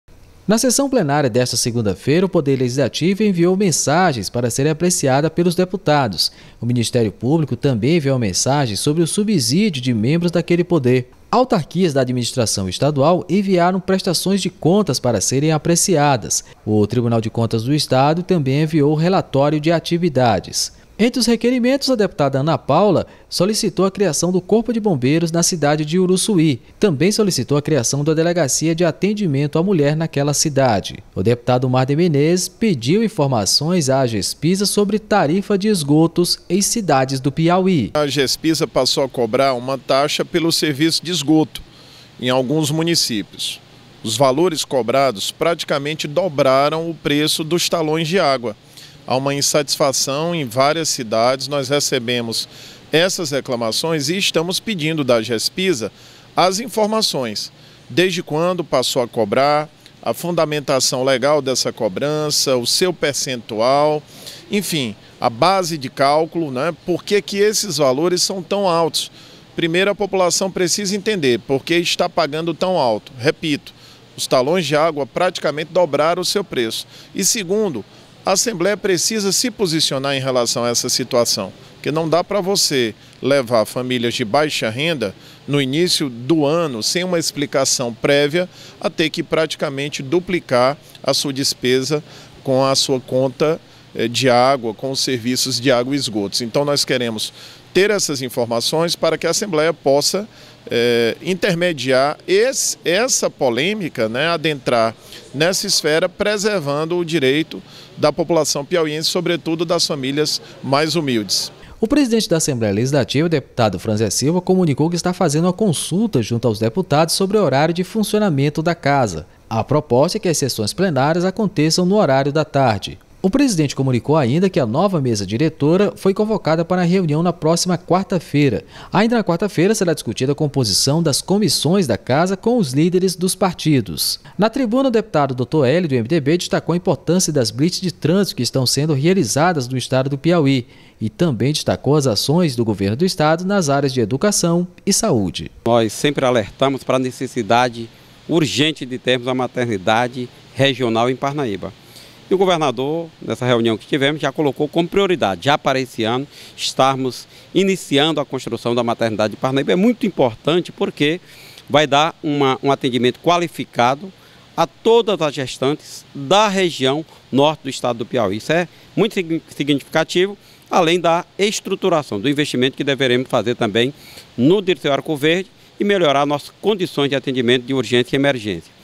Sessão Plenária 06.02.23 - Deputados discutiram sobre ações realizadas pelos governo do Estado
Na primeira sessão plenária da semana, os deputados estaduais discutiram sobre ações realizadas pelo governo do Estado nas áreas da educação e da saúde, e solicitaram mais investimentos para o interior do Piauí. Mais informações na reportagem de James Almeida e  Luís Carlos Paiva, da TV Assembleia.